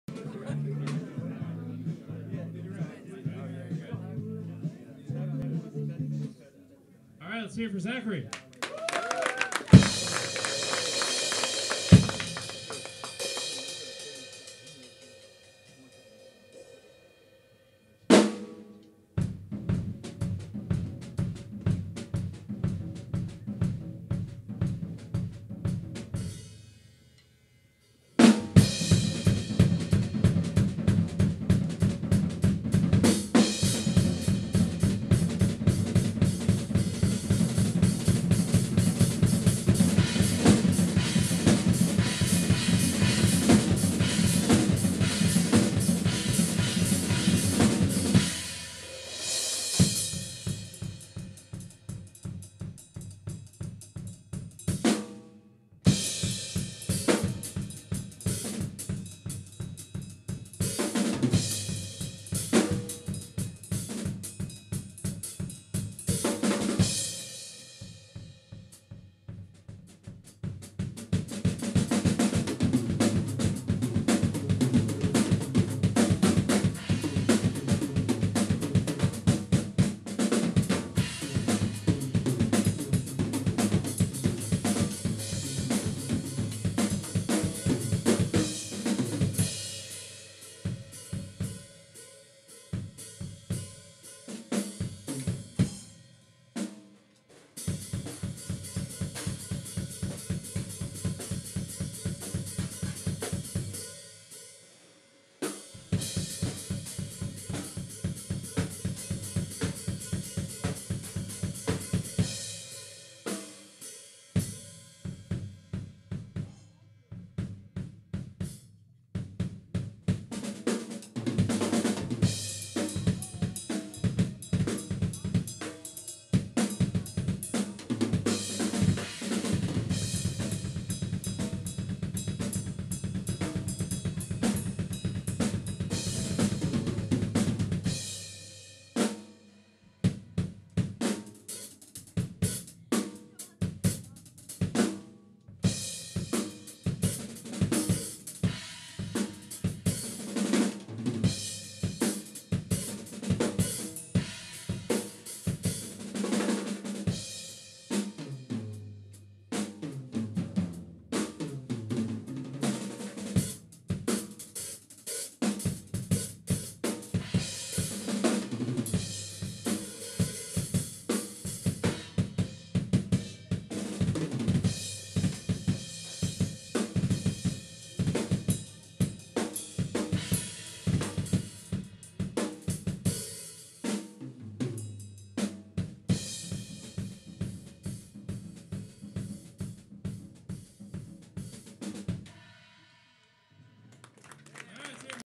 GC Drum Off Preliminary
This the solo I played at my local Guitar Center's Drum Off 2012 preliminary round. I ended up winning my night with an epic tie breaker for the other spot to advance. I went on to compete at store finals.